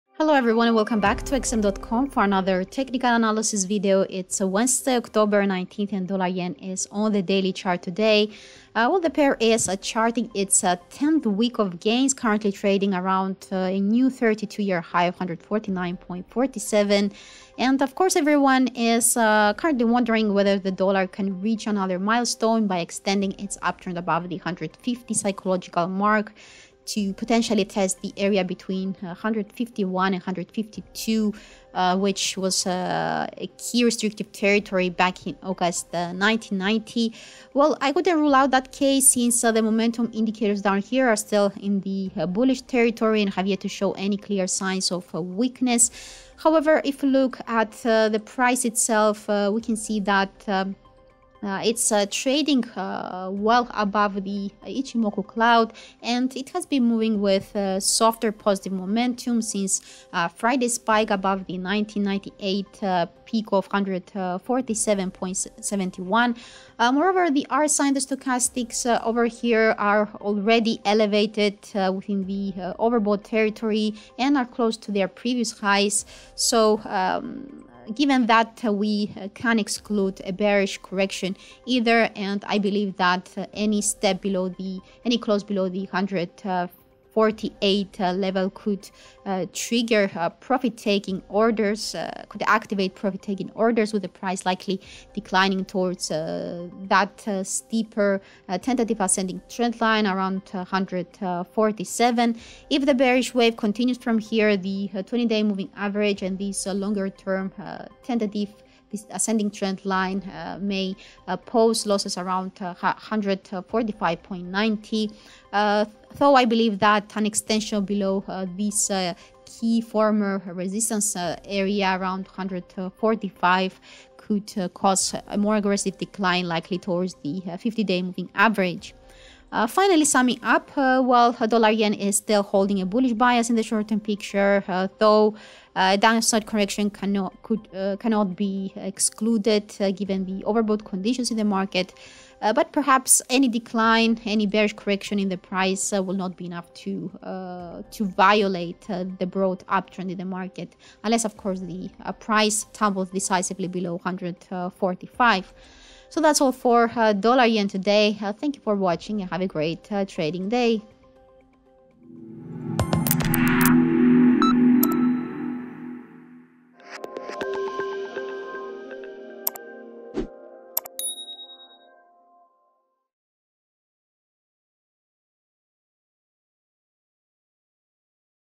Technical Analysis: 19/10/2022 - USDJPY aims for 150; overbought signals strong
For more information on the topics discussed in this video, including detailed charts and tables, read the full Technical Analysis article: